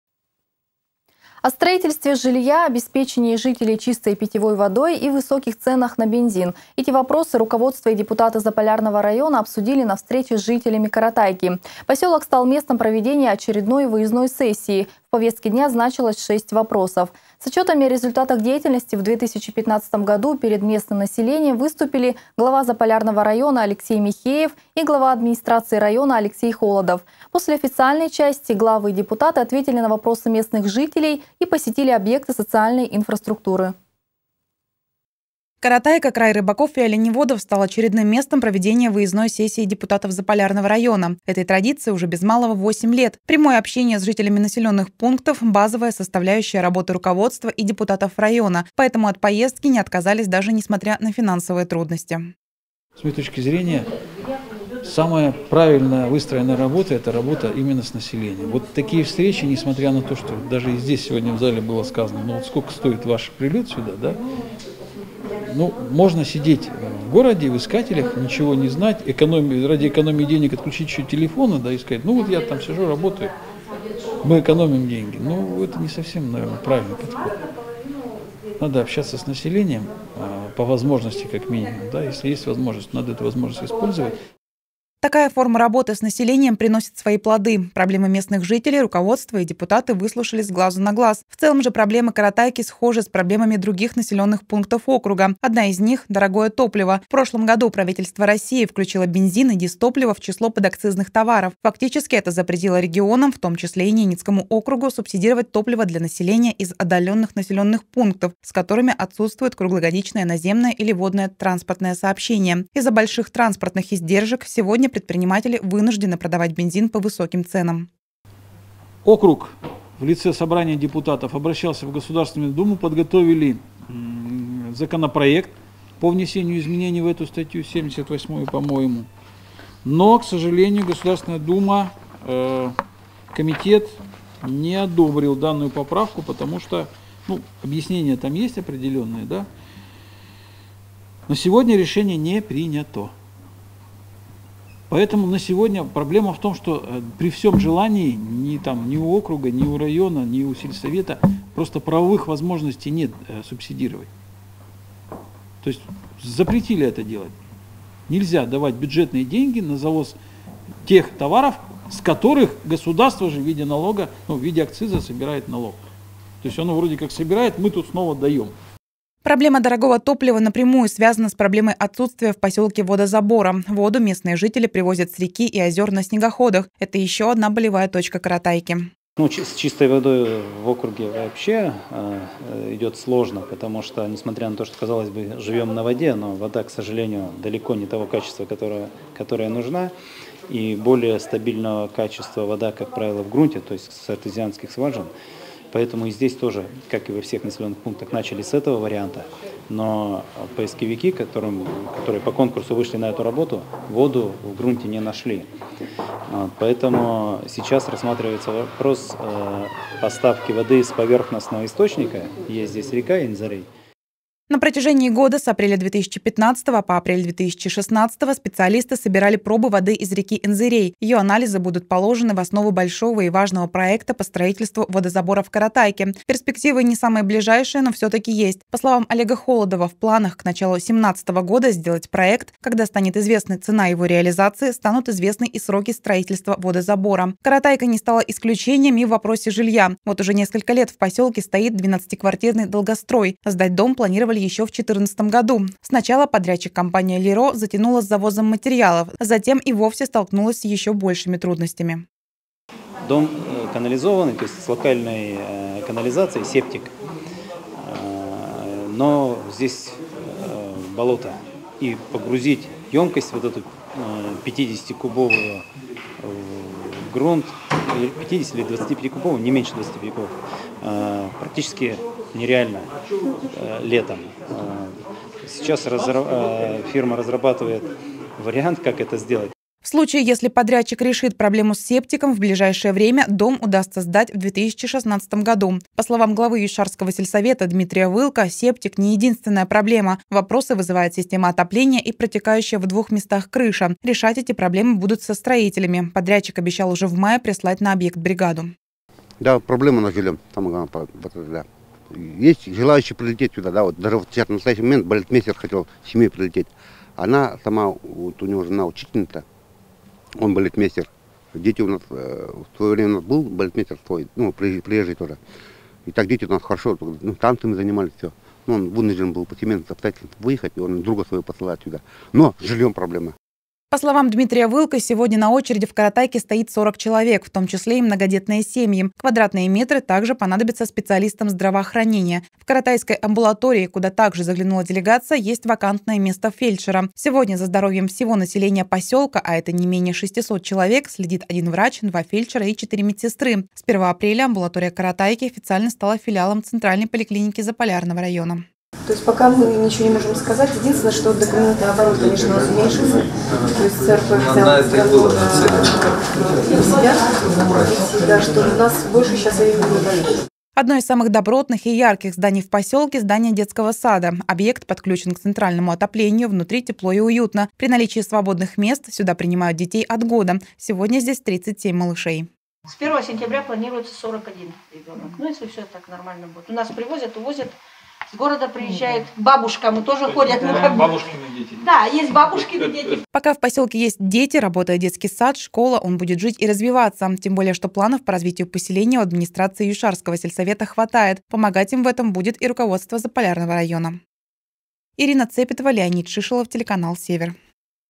Депутаты Заполярного района провели выездную сессию в Каратайке
О строительстве жилья, обеспечении жителей чистой питьевой водой, проблеме безработицы и пьянства. Эти вопросы руководство и депутаты Заполярного района обсудили на встрече с жителями Каратайки. Поселок стал местом проведения очередной выездной сессии. В повестке дня значилось 6 вопросов. С отчетами о результатах деятельности в 2015 году перед местным населением выступили глава Заполярного района Алексей Михеев и глава Администрации района Олег Холодов.

Каратайка — край рыбаков и оленеводов — стал очередным местом проведения выездной сессии депутатов Заполярного района. Этой традиции уже без малого 8 лет. Прямое общение с жителями населенных пунктов — базовая составляющая работы руководства и депутатов района. Поэтому от поездки не отказались даже несмотря на финансовые трудности.

Согласно регламенту выездная сессия предполагает подведение итогов и результатов деятельности. Вот и в это раз руководство района отчиталось перед местными жителями о проделанной работе в 2015 году и наметило планы на ближайшую перспективу. После официальной части, главы и депутаты ответили на вопросы местных жителей и посетили объекты социальной инфраструктуры.